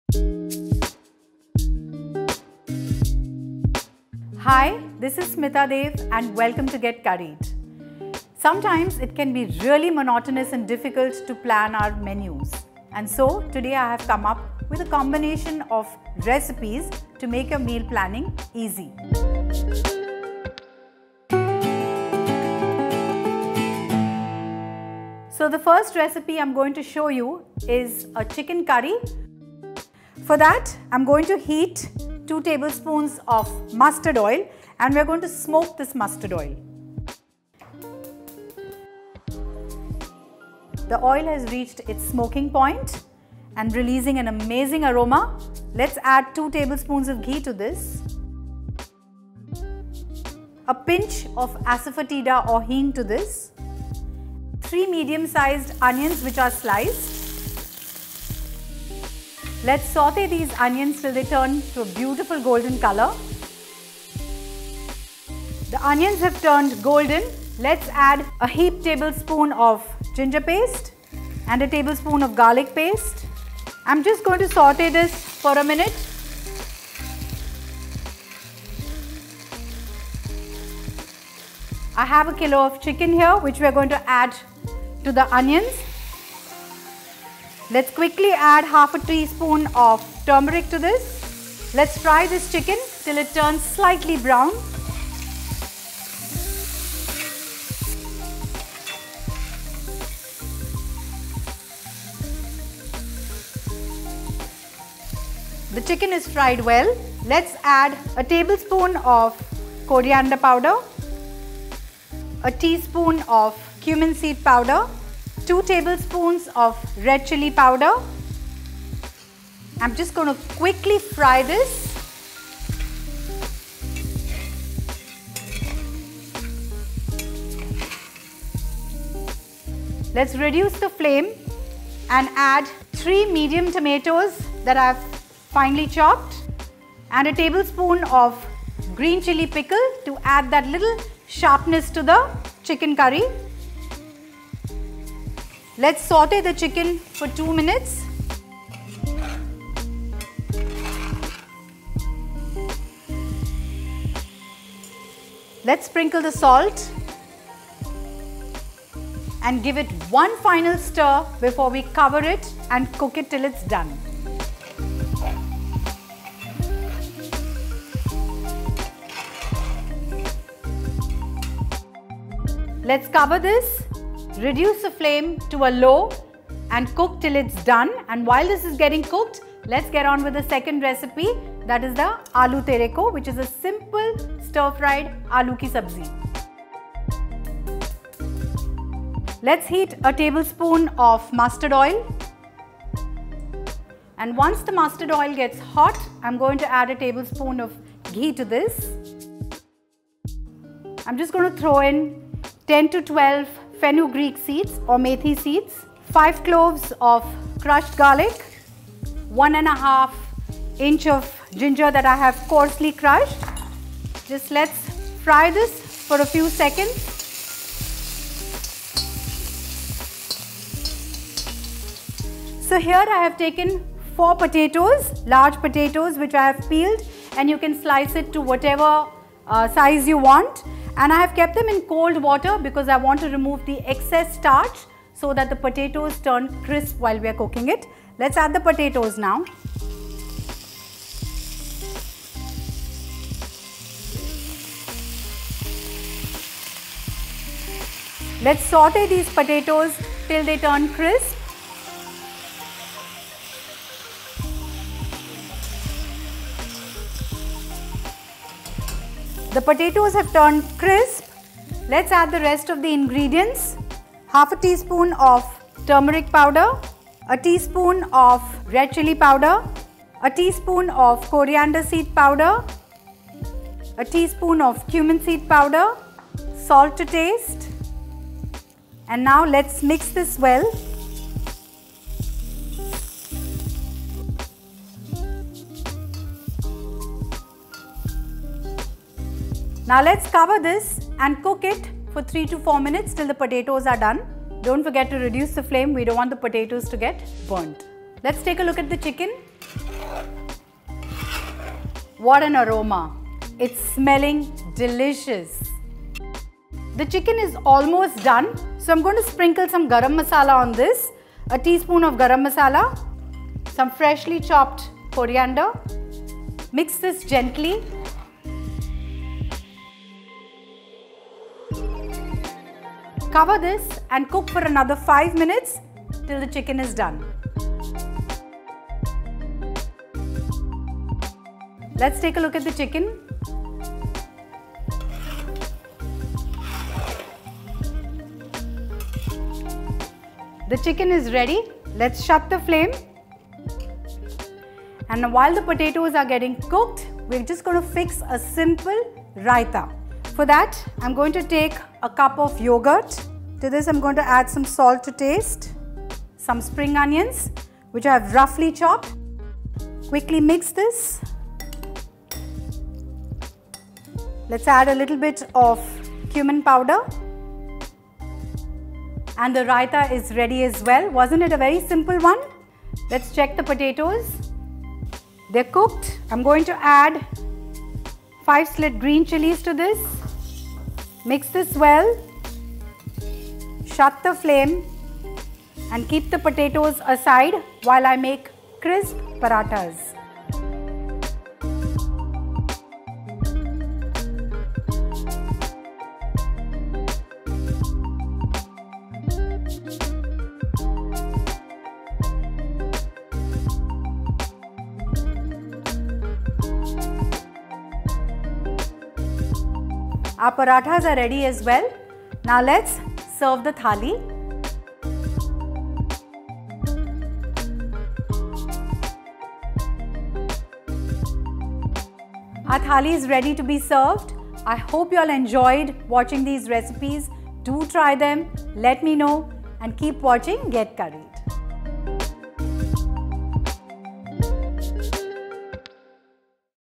Indian Mini Thali | Easy Lunch Thali | Easy Indian Meal Preps | Smita Deo Recipes
Learn how to make Indian Mini Thali with our Chef Smita Deo.

Introduction
Thali (meaning "plate") or Bhojanam (meaning "full meal") is a round platter used to serve food in South Asia, Southeast Asia, and the Caribbean. Thali is also used to refer to an Indian-style meal made up of a selection of various dishes which are served on a platter. The Easy Lunch Thali is a mini-meal that includes- fried chicken curry, aloo tareko, simple raita & paratha. Do try this simple thali recipe & let us know how you like it in the comments below.

Indian Mini Thali Ingredients :

For Chicken Curry
2 tbsp Mustard Oil
2 tbsp Ghee
1/8 tsp Asafoetida
3 Onions (sliced)
1 tbsp Ginger Paste
1 tbsp Garlic Paste
1 kg Chicken
1/2 tsp Turmeric Powder
1 tbsp Coriander Seed Powder
1 tsp Cumin Seed Powder
2 tbsp Red Chilli Powder
3 Tomatoes (chopped)
1 tbsp Green Chilli Pickle
Salt (as required)
1 tsp Garam Masala Powder
2 tbsp Coriander Leaves (chopped)

For Aloo Tareko
1 tbsp Mustard Oil
1 tbsp Ghee
10-12 Fenugreek Seeds
5 Garlic Cloves (crushed)
1 & 1/2-inch Ginger (crushed)
4 Potatoes (sliced)
1/2 tsp Turmeric Powder
1 tsp Red Chilli Powder
1 tsp Coriander Seed Powder
1 tsp Cumin Seed Powder
Salt (as required)
5 Green Chillies (slit)

Simple Raita
1 cup Yogurt
Salt (as required)
1/2 cup Spring Onions (chopped)
1/4 tsp Cumin Powder

Host: Smita Deo
Copyrights: REPL